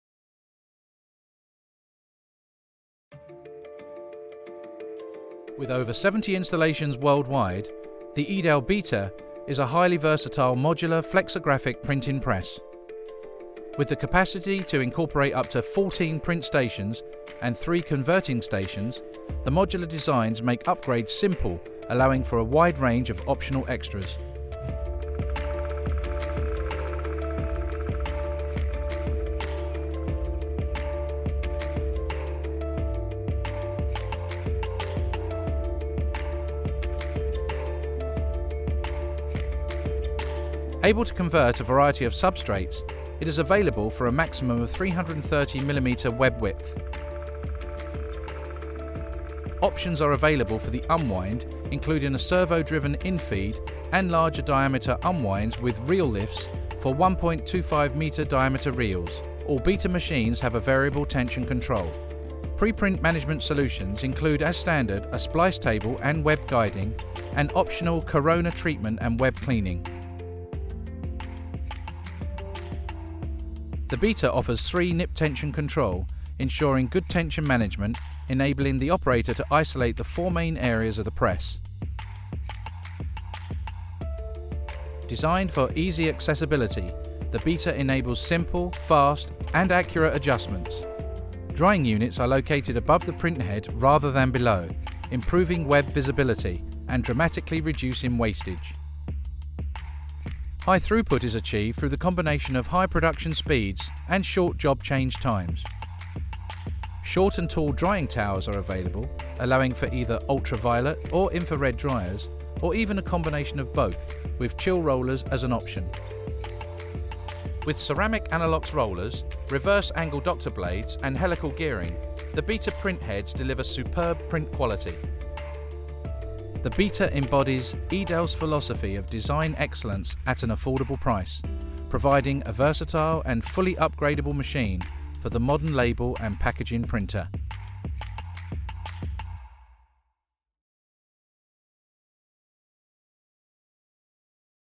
Узкорулонная флексографская печатная машина Edale Beta
Александр Онищук, руководитель отдела продаж
флексографских систем MacHOUSE:
«Английская компания Edale занимается разработкой и производством печатного оборудования уже более 60 лет. Сегодня Edale является одним из ведущих мировых производителей узкорулонных флексографских машин с шириной печати от 250 мм до 510 мм, предназначенных для печати самоклеящихся этикеток, упаковки и картона. Машины этого производителя пользуются популярностью и на украинском рынке. Основные преимущества оборудования Edale - высокая производительность и использование всех существующих технологий по изготовлению и отделке продукции (персонализация, тиснение, ламинирование, использование защитных бумаг и красок и т.д.). Высокий уровень автоматизации машин позволяет свести к минимуму их техническое обслуживание».